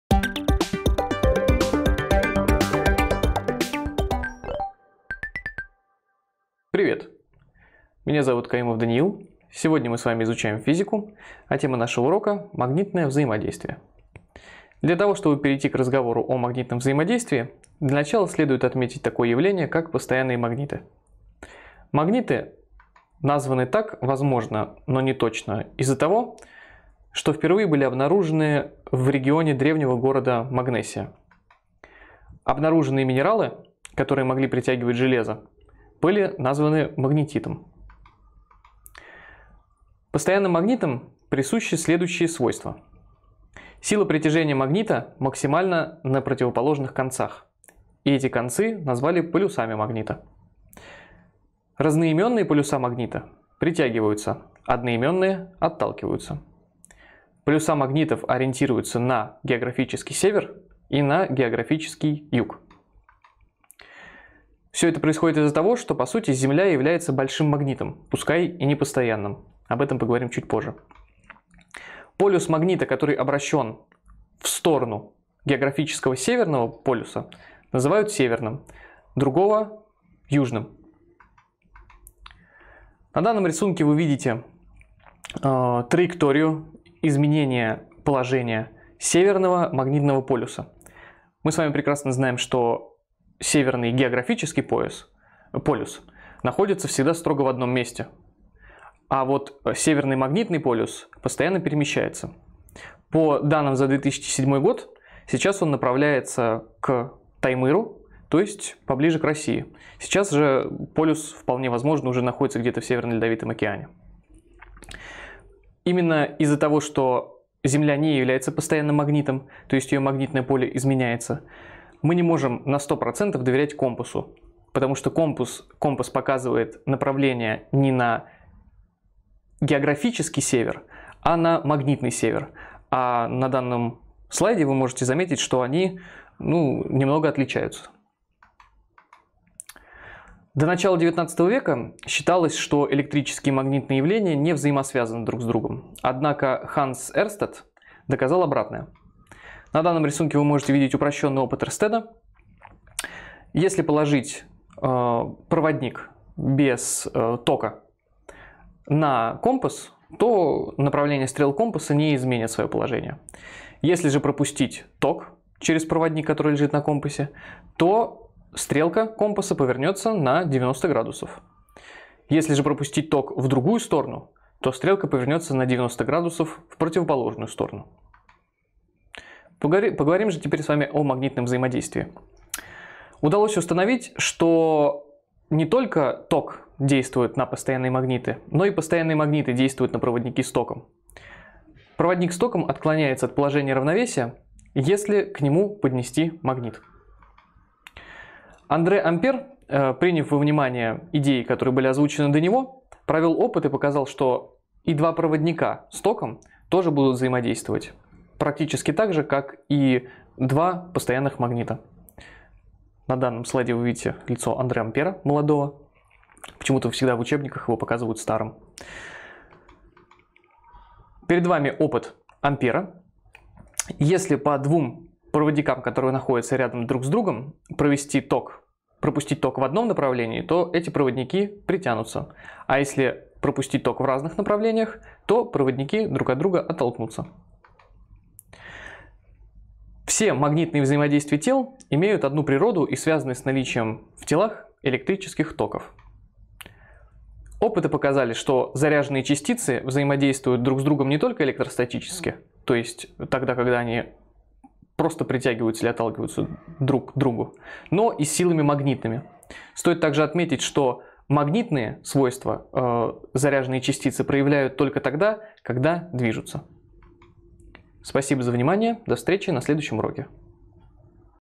Урок 9. Магнитное взаимодействие. Физика 11 класс
Тема сегодняшнего урока – Магнитное взаимодействие. В этом уроке мы познакомимся с такими понятиями, как постоянный магнит и магнитное взаимодействие, рассмотрим опыты Андре Ампера, а также узнаем, почему нельзя доверять компасу.

Онлайн-школа №1 - это первая настоящая общеобразовательная
дистанционная школа с аттестатом и программой ФГОС.